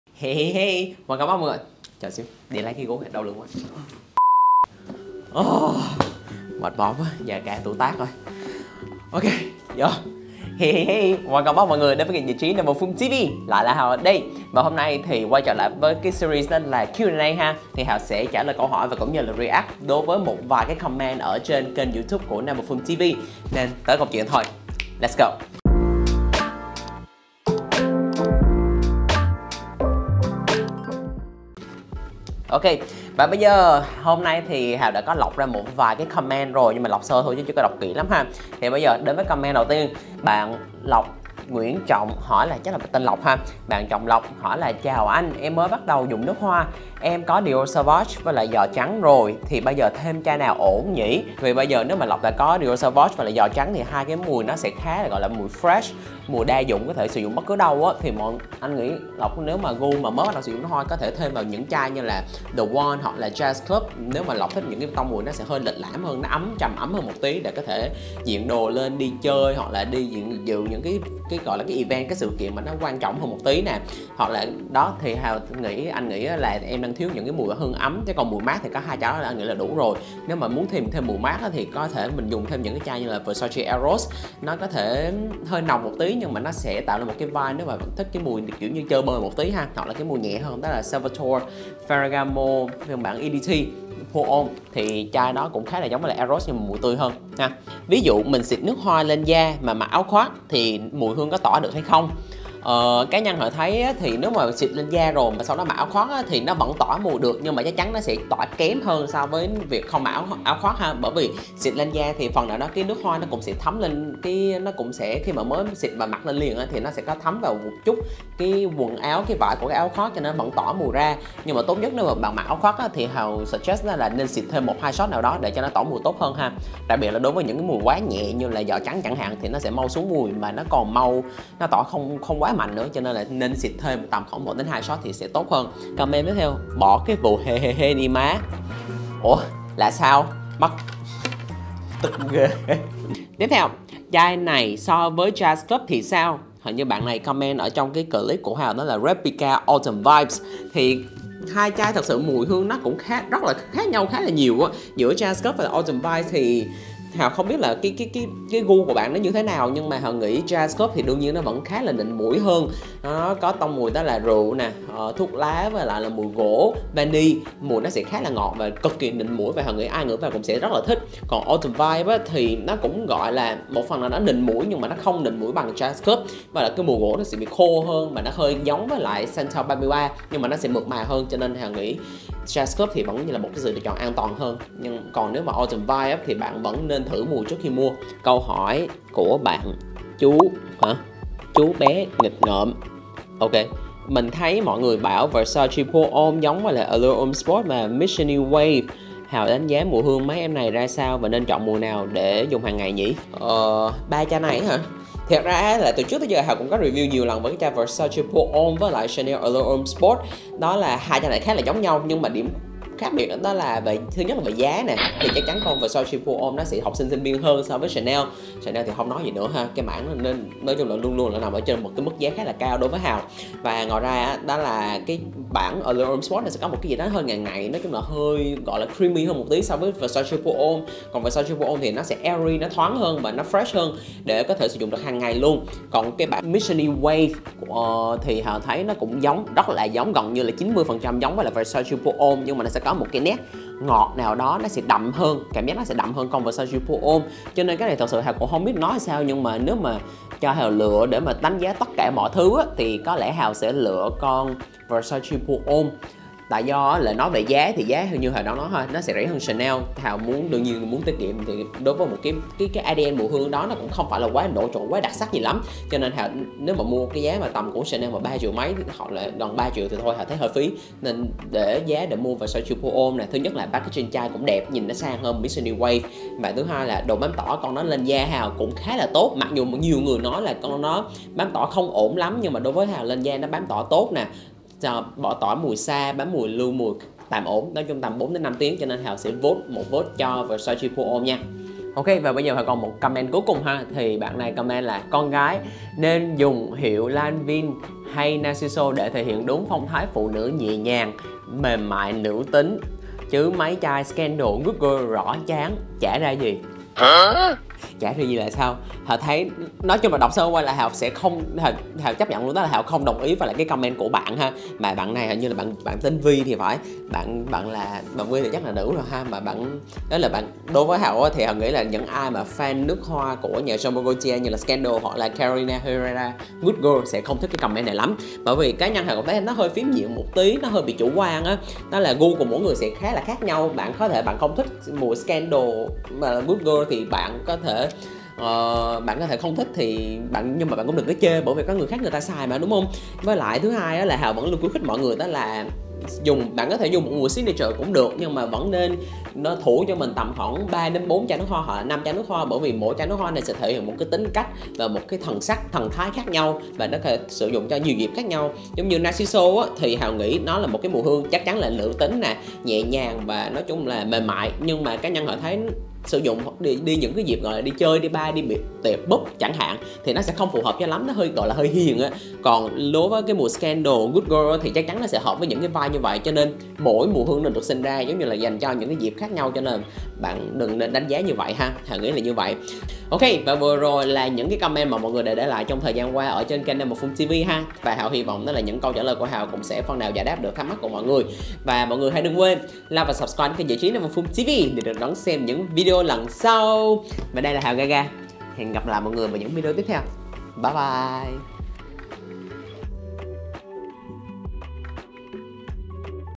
Q&A: Con gái dùng nước hoa Narciso mới là nữ tính??? | namperfume TV
Con gái không dùng nước hoa Narciso Rodriguez thì không nữ tính? What!!!?? Đây là một trong những bình luận độc đáo và đầy tranh cãi Hào đã lượm được ở trên kênh của namperfume TV. Mọi người hãy xem hết video còn comment nào độc đáo như vậy không nhé